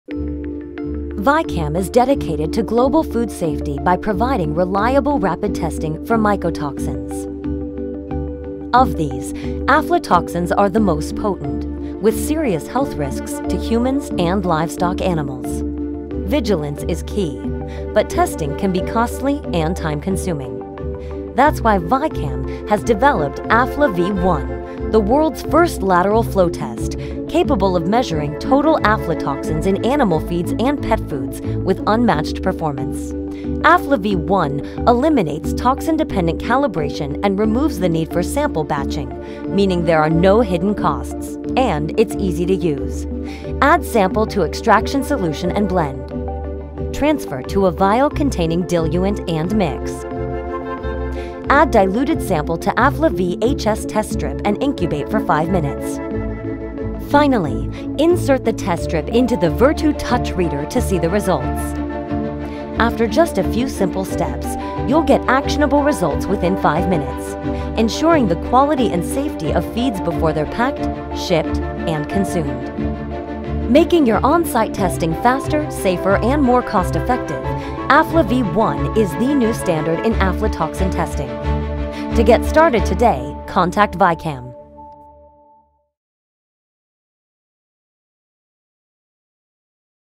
Afla-V ONE: Safe, Sustainable Aflatoxin Detection for Complete Feeds and Pet Foods
VICAM's Afla-V ONE introduces a NEW approach to aflatoxin detection and management for animal feed and pet food producers worldwide.  Obtain fast, actionable results in just minutes, while improving safety and sustainability in the testing and manufacturing environment.  Explore the future of aflatoxin detection, today.